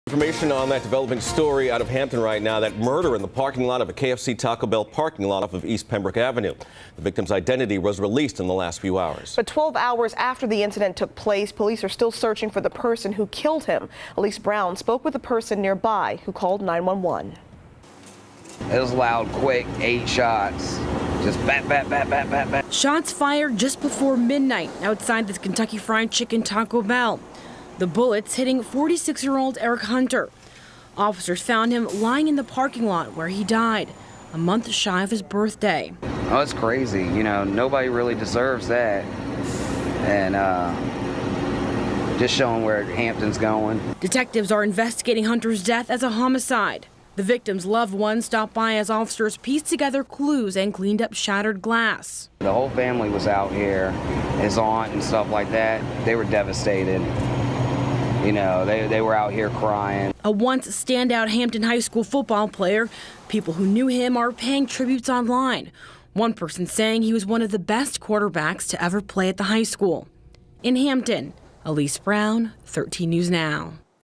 Hampton KFC-Taco Bell Murder
That murder in the parking lot of a K.F.C-Taco Bell parking lot off East Pembroke Avenue. The victim's identity was released.